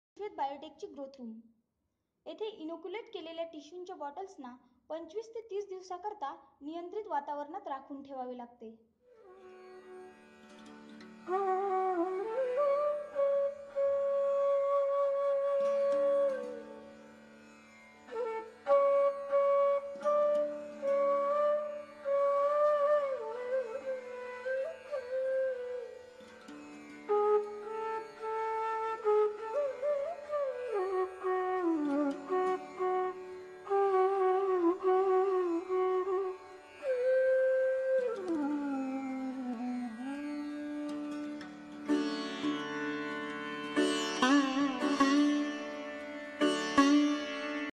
ISHVED Biotech Growth Rooms
Our inoculated culture gets proper caring under Four growth rooms having capacity to accommodate 7,00,000 culture bottles at any time of consideration within a year.
Our growth rooms have provision to maintain all physical factors on automatic functioning.
We ascertain all the epigenetic control during in vitro growth of each single culture to overcome the promise for genetically uniform plants